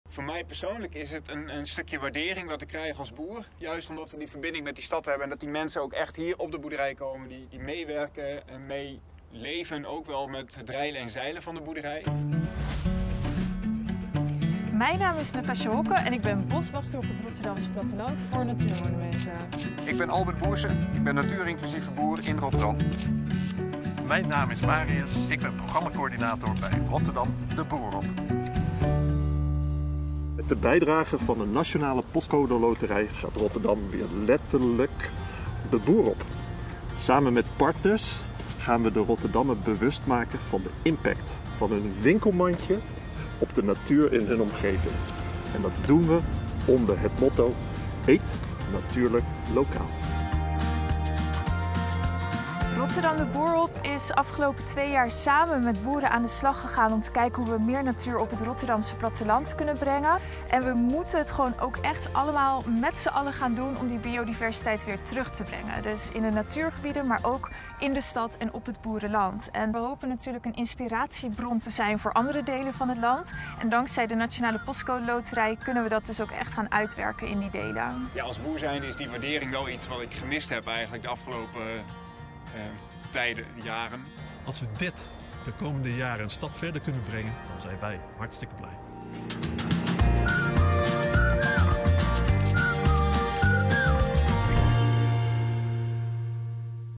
Nationale Postcode Loterij steunt Natuurmonumenten met Rotterdam de boer op!
Ter gelegenheid van de toekenning van maar liefst 8,8 miljoen van de Nationale Postcode Loterij aan Natuurmonumenten voor Rotterdam de boer op! maakte de loterij dit filmpje met boer Albert Boersen, boswachter Natascha Hokke en programmamanager Marius Brants.